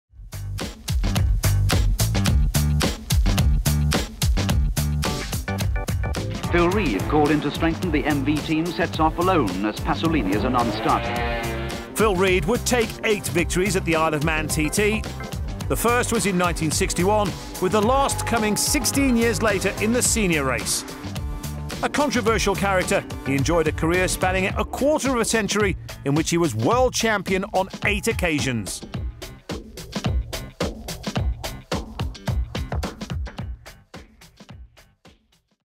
TT Launch 2018 - Q - Phil Read
Riding everything from AJS and Norton singles to exotic Yamahas and MV Agustas Phil Read scored  eight TT wins. Is he your number one?

(entries close 31st March, 2018)